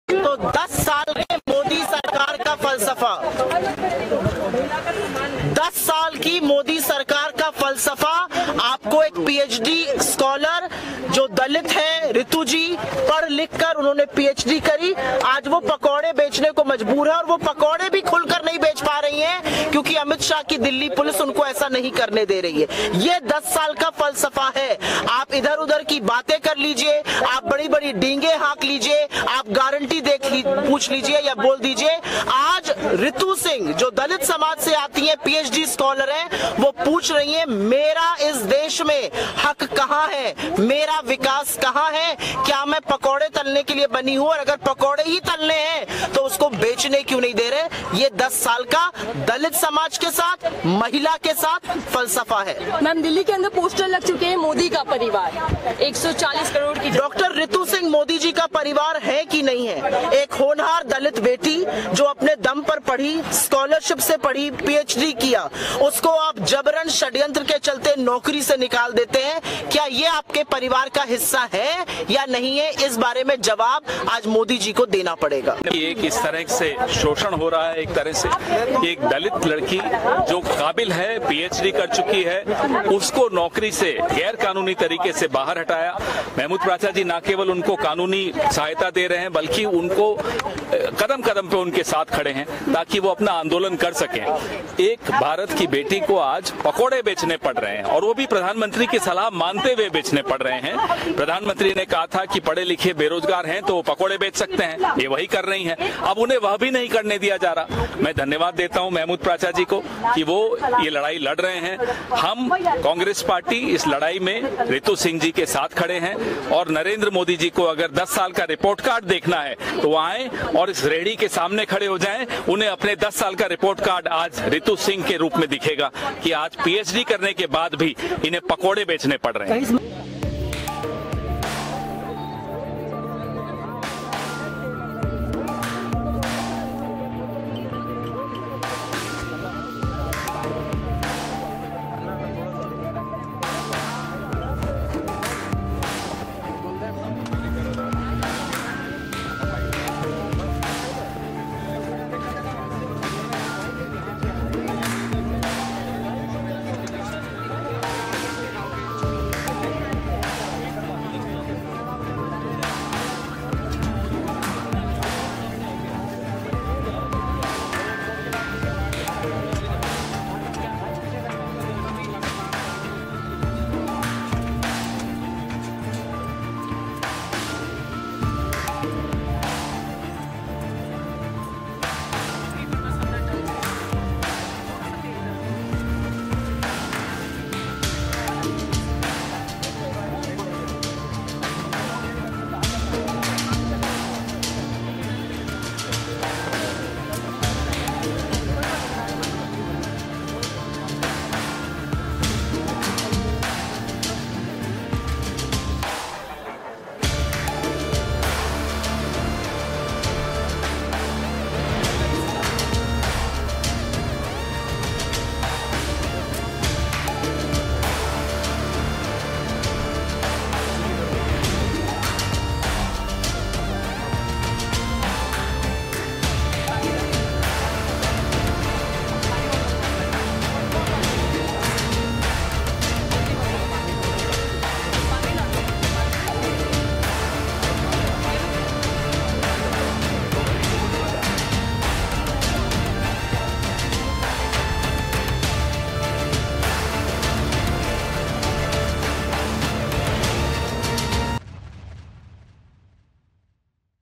Delhi News: नौकरी से निकाले जाने पर पूर्व प्रोफेसर ने DU के सामने लगाई पकौड़े की रेहड़ी | Aaj Tak
डॉ. रितु सिंह पहले दिल्ली विश्वविद्यालय के दौलत राम कॉलेज में मनोविज्ञान विभाग में एडहॉक प्रोफेसर रह चुकी हैं. वो करीब एक साल तक असिस्टेंट प्रोफेसर के पद पर रहीं. रितु का आरोप है डीयू प्रशासन ने दलित होने की वजह से उन्हें नौकरी से निकाल दिया.